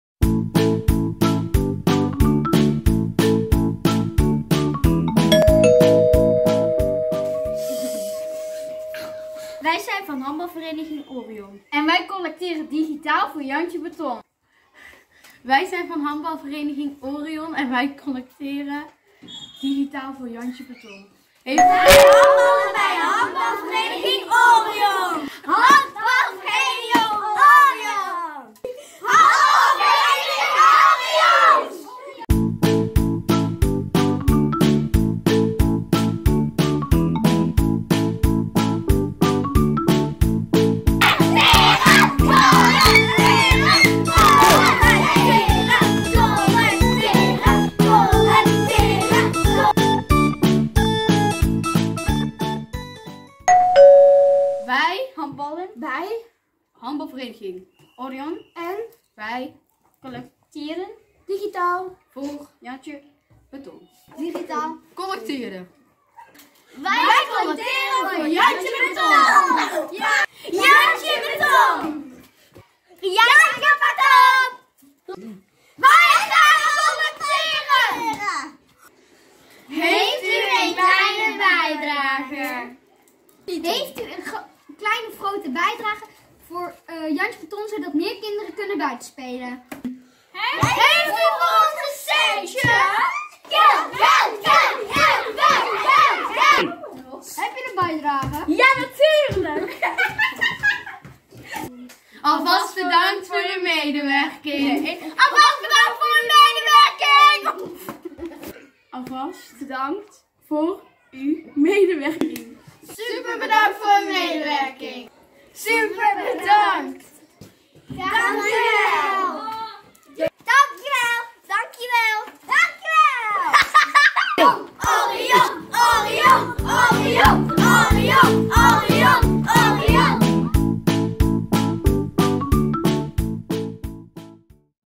Digitale collectie JantjeBeton 2021
Ook dit jaar gaan we weer collecteren voor dit goede doel, waarmee we ook een centje verdienen voor onze eigen clubkas. Deze collecte vindt plaats in de week van 8 t/m 13 maart.

Vanwege Corona collecteren we dit jaar niet huis aan huis, maar met de digitale collectebus! Dit betekent dat we alle leden weer hard nodig hebben in het doordelen van de digitale collectebus via social media.